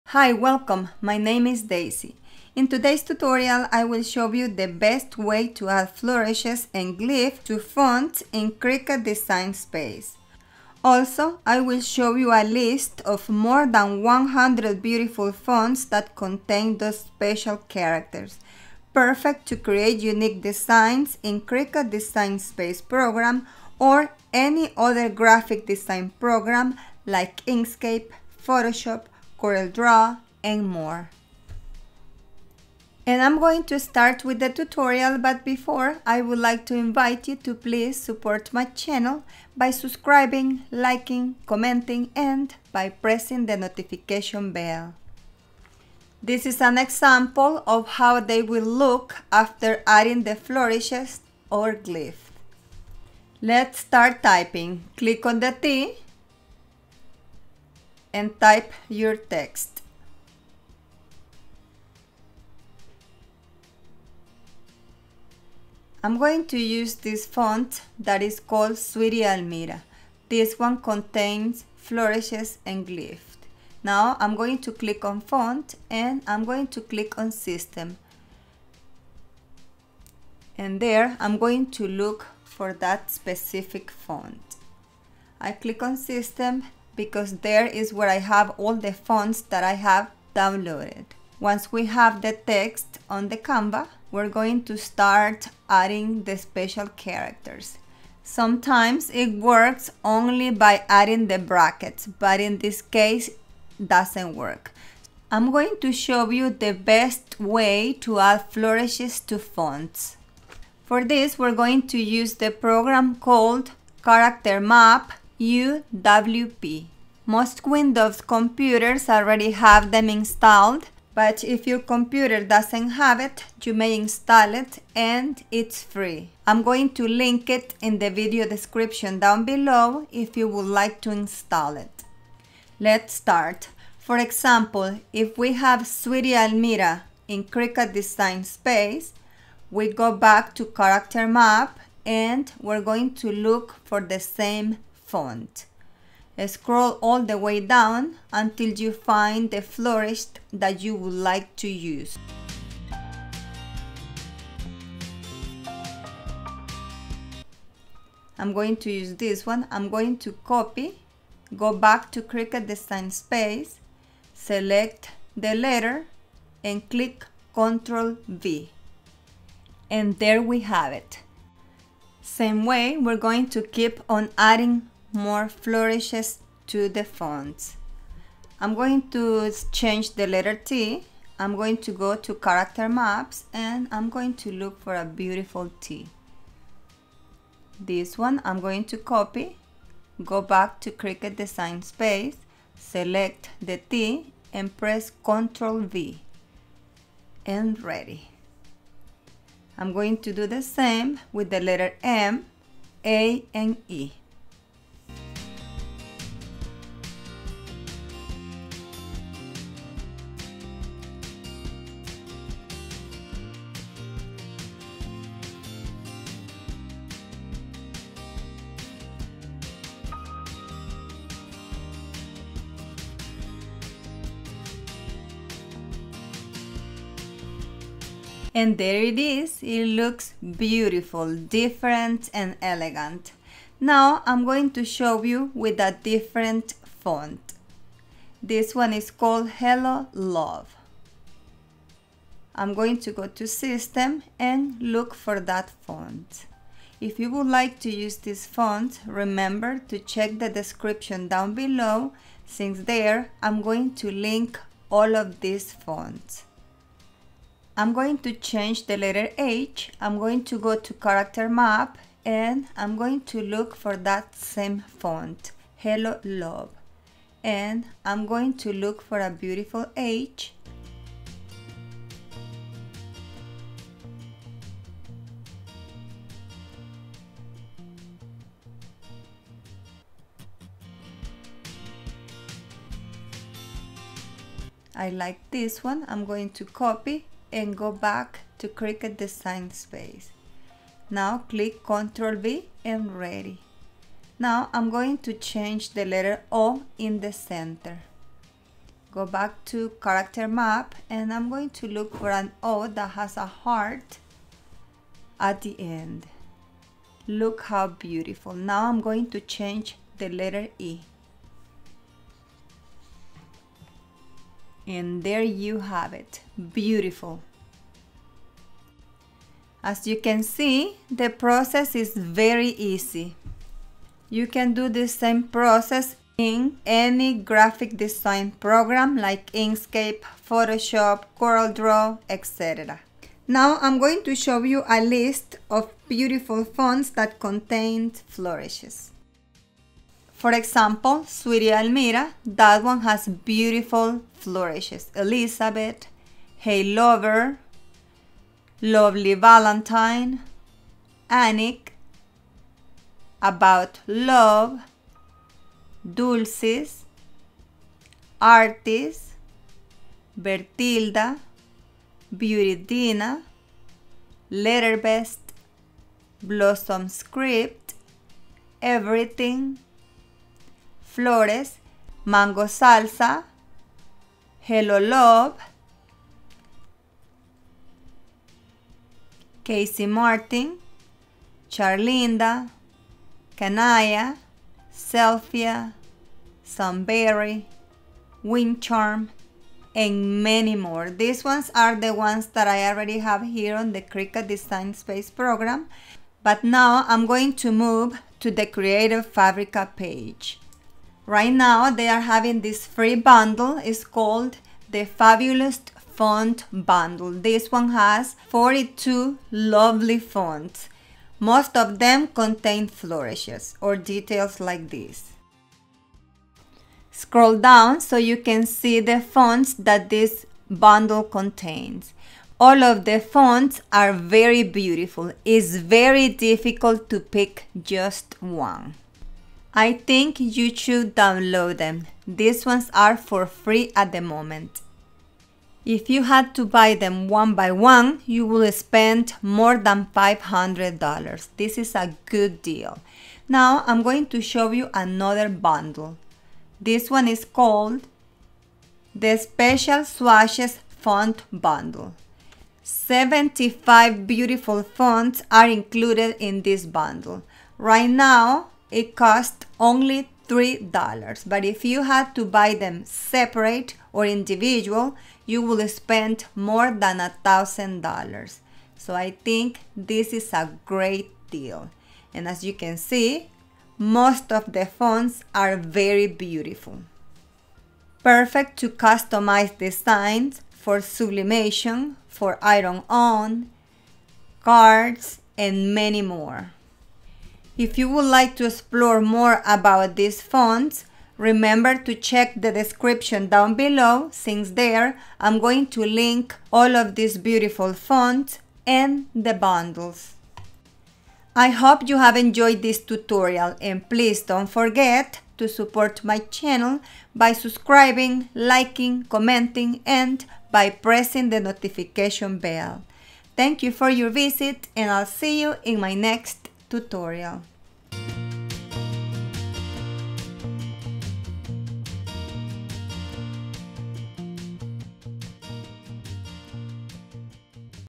How to ADD Flourishes and GLYPHS to Fonts in CRICUT DS | List of 100+ Font with Glyphs
Step-by-step tutorial on how to add GLYPHS, FLOURISHES, CURLS and details to fonts in the Cricut Design Space program. You can implement the same process in other graphic design programs such as Inkscape, Photoshop, Corel Draw, and more.

In addition, I will provide you with a list of more than 100 fonts with glyphs, flourishes curls and details that you could download from Creative Fabrica.